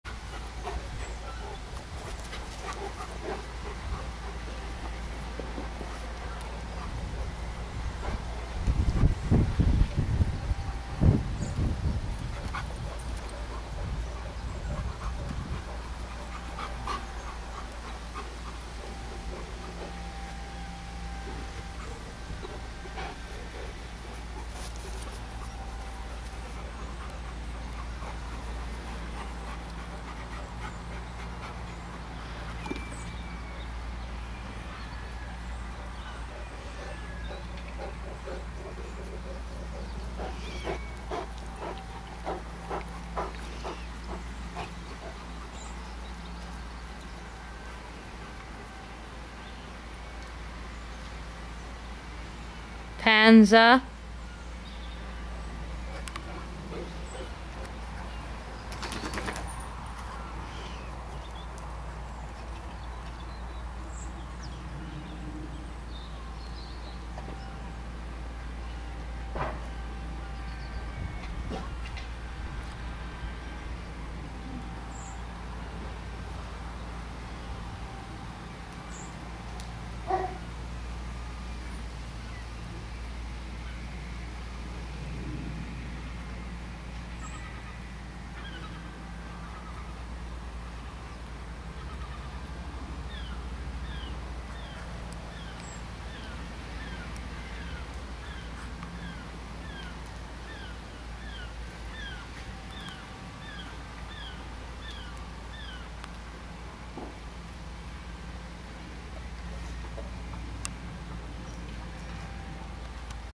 Bear white shepherd his first day in the mastiff pack(7)
Bear white shepherd his first day in the mastiff pack